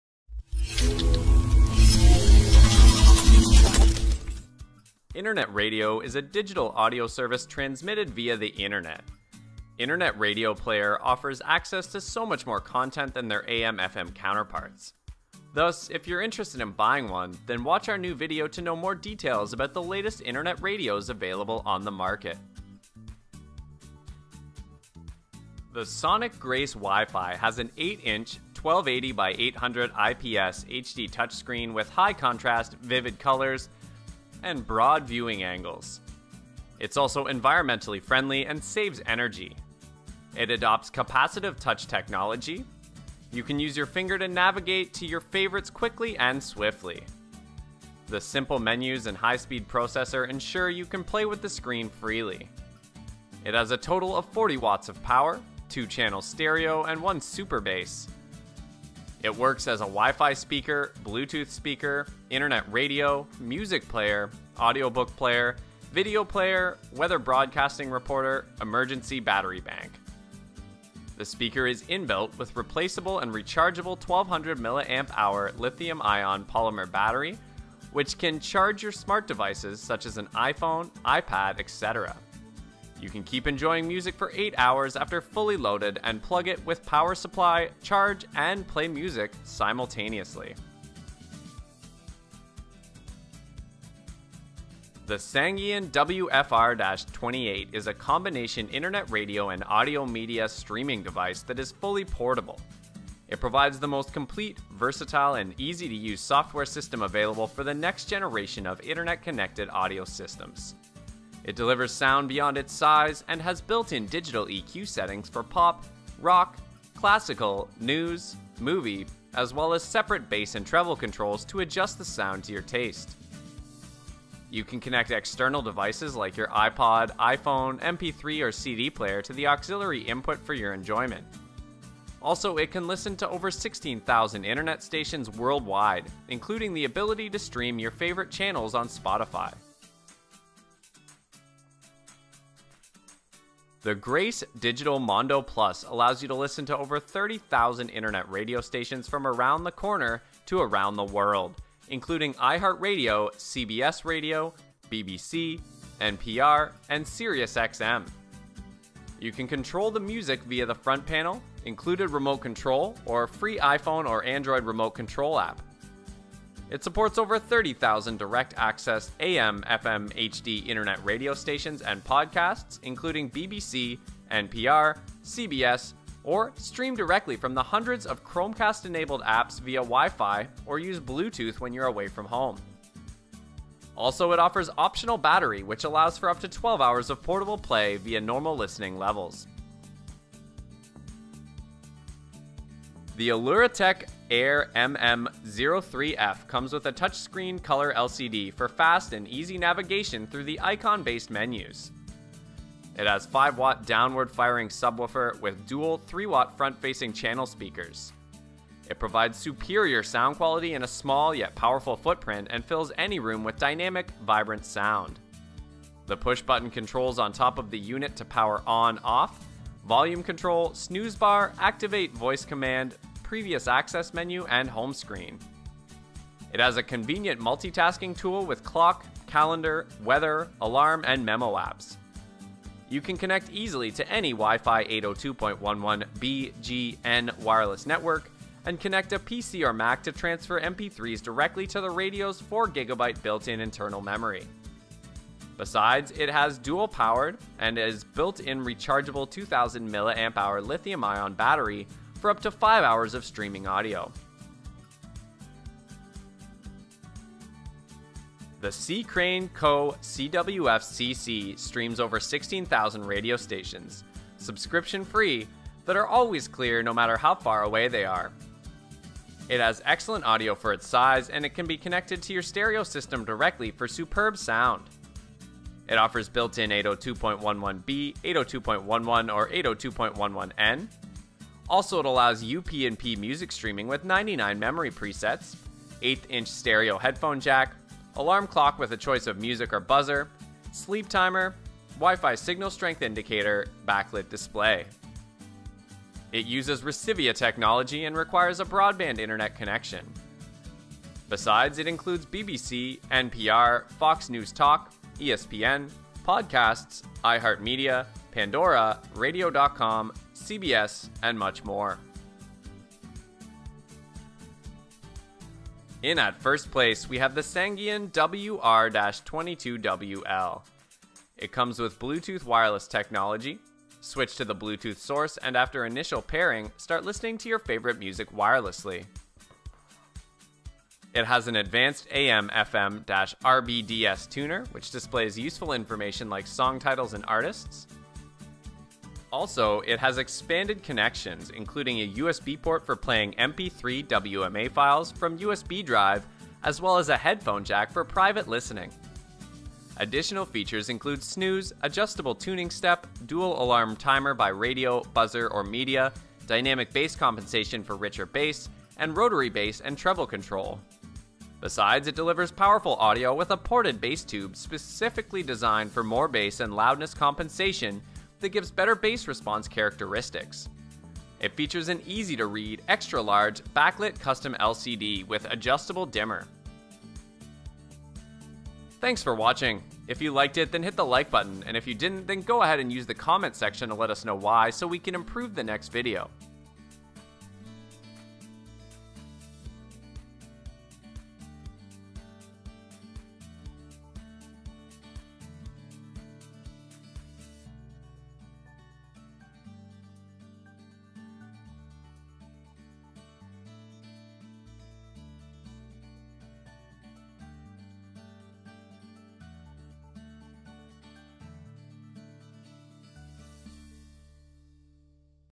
6 Best Internet Radio Reviews
List products in video. Click the link below check on Amazon:

1. Sangean WR-22WL:

2. C Crane Co CWF CC:

3. Aluratek AIRMM03F:

4. Grace Digital Mondo+:

5. Sangean WFR:

6. Sonicgrace WIFI: